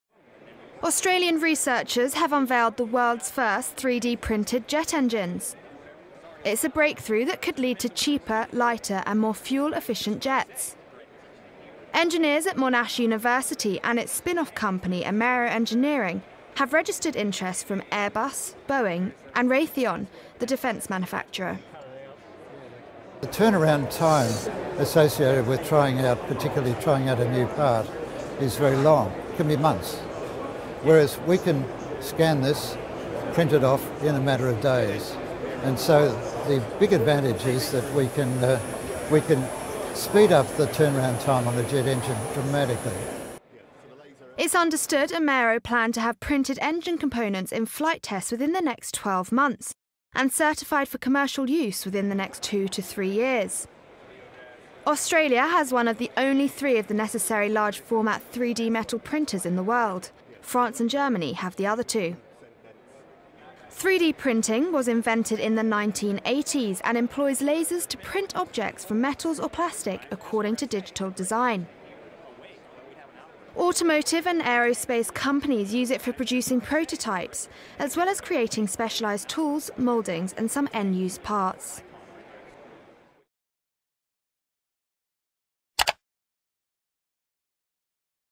World's first 3D printed jet engine is unveiled
Australian researchers unveil the world's first 3D printed jet engine that could lead to cheaper, lighter and more fuel-efficient jets. Report by Gabriella Darlington.